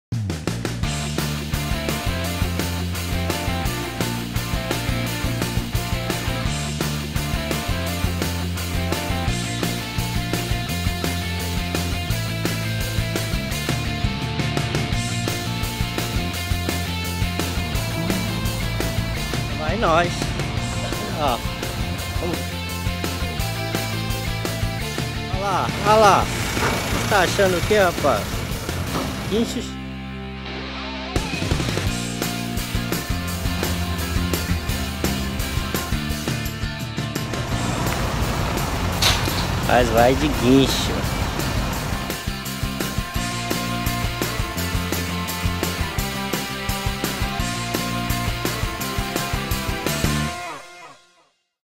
仕事の帰りの出来事
仕事の帰りにバイク故障してしまった。
クラッチワイヤー切れた。